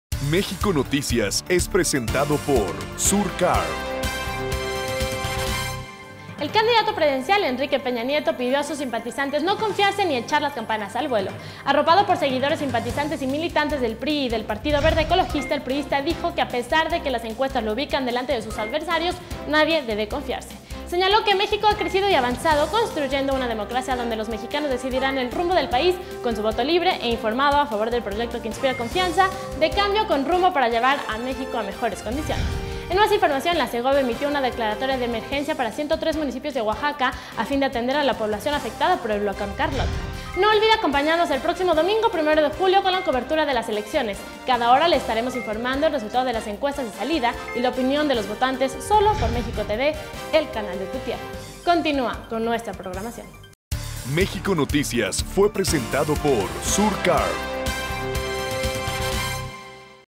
EPN pidió a sus simpatizantes no confiarse
El candidato presidencial, Enrique Peña Nieto, pidió a sus simpatizantes no confiarse ni echar las campanas al vuelo. Arropado por sus seguidores el priista dijo que a pesar de que las encuestas lo ubican delante de sus adversarios, nadie debe confiarse.